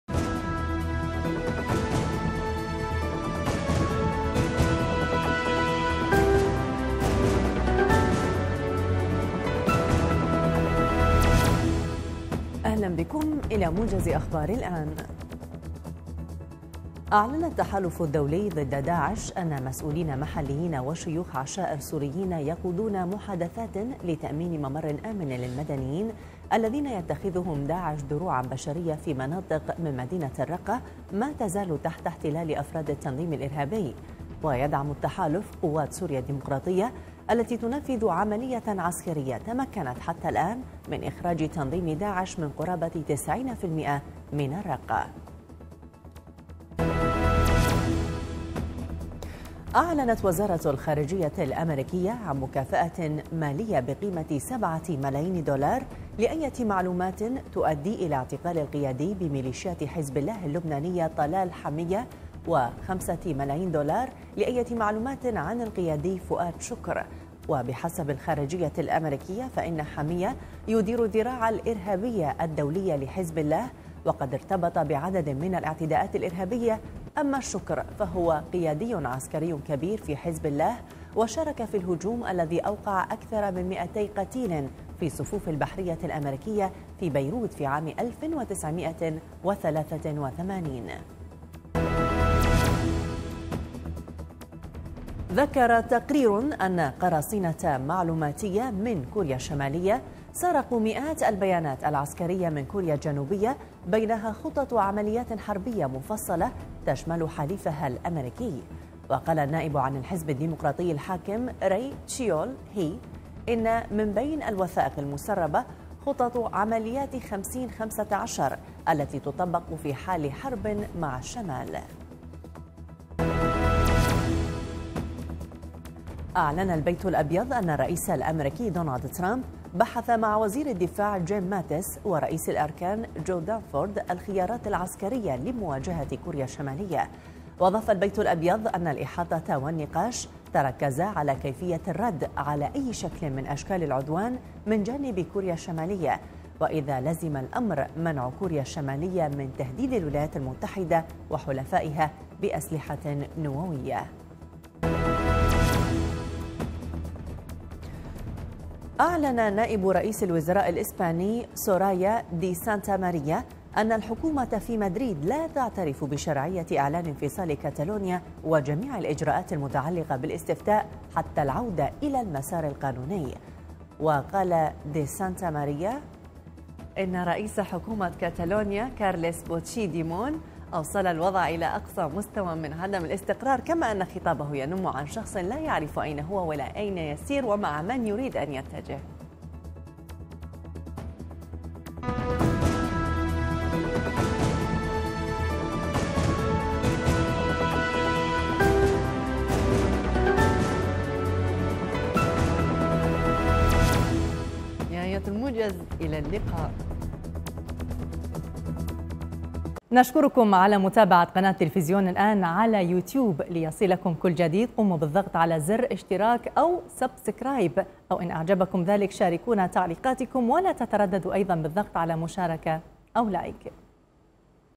11-10-2017 | موجز التاسعة صباحاً.. لأهم الأخبار من
أبرز العناوين:
- محادثات لإخراج المدنيين من مناطق احتلال داعش في
- واشنطن ترصد مكافأة مالية لإعتقال اثنين من قادة حزب الله اللبناني
- كوريا_الشمالية قرصنت خططا حربية لجيش
- ترامب بحث مع القادة العسكريين الخيارات في مواجهة كوريا الشمالية